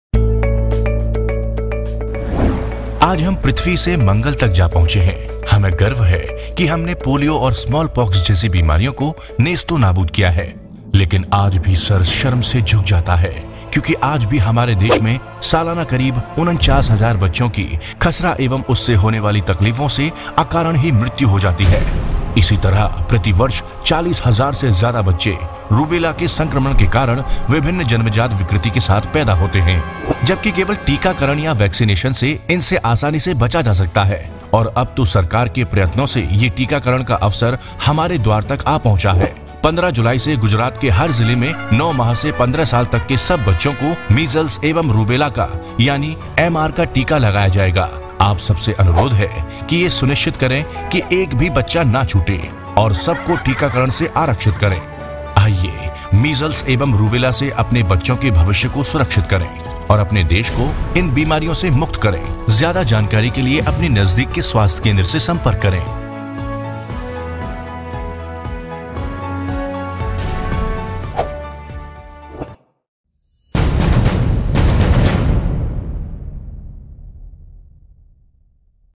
MR Vaccination CAMPAIGN HINDI Promotional Why should ALL children take this M R Vaccine
A promotional video by Gujarat Govt - all rights are with owners, video being share for parent education